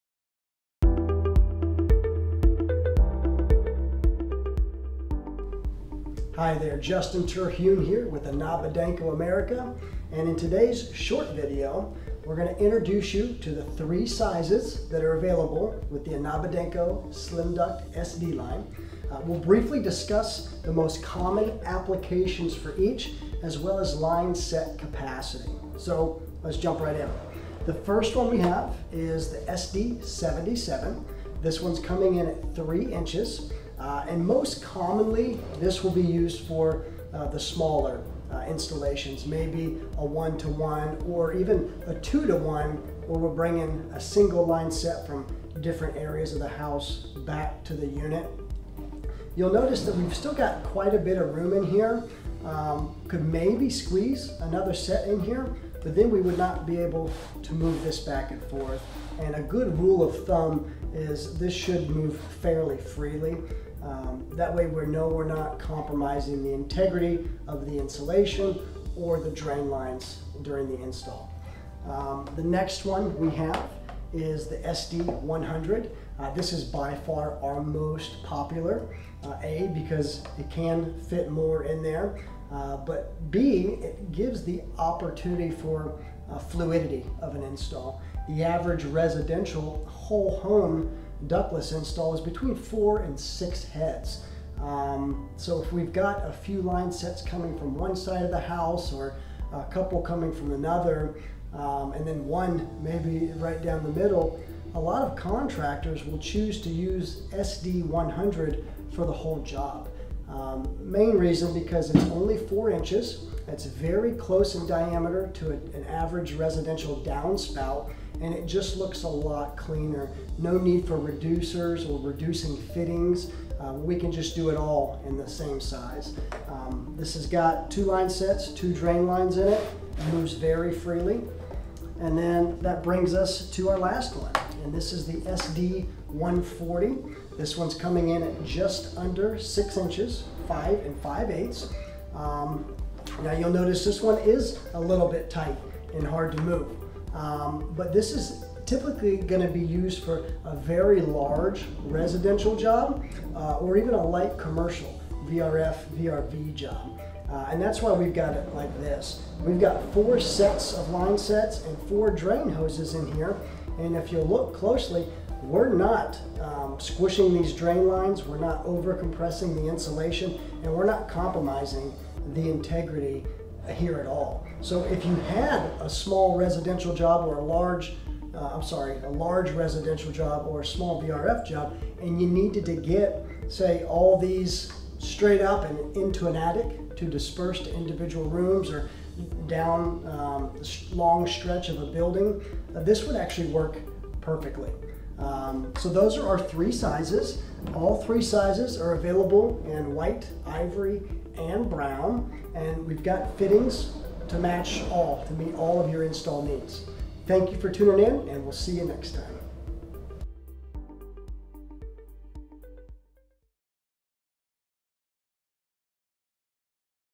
Lineset Cover Tips & Tricks - Slimduct Lineset Capacity
Learn lineset cover installation tips and tricks for the field and how our Slimduct lineset covers can make installations easier and sturdier. For more product info, visit rs.today/slimduct

Follow us for new product releases and sign up for our newsletter!